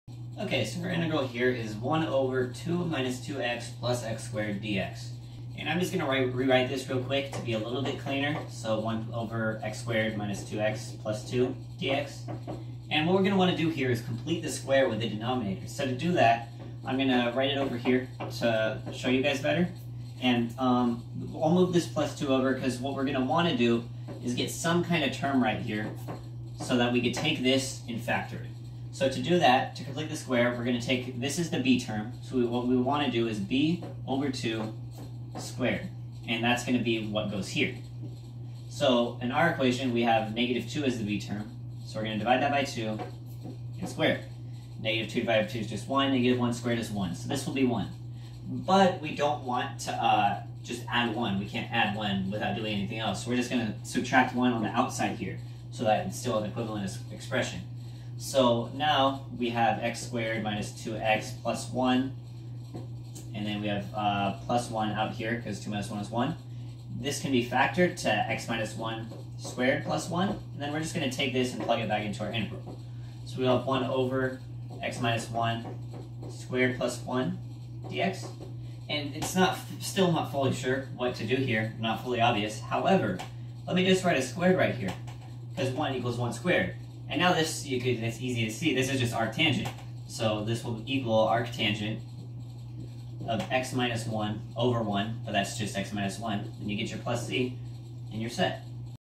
UNSW Integration Bee 2022 - Round 1 #3 | Math Mondays #3
Will I be able to solve every problem from all the integration bees I get these from? Probably not. Will I try? You can bet your life on it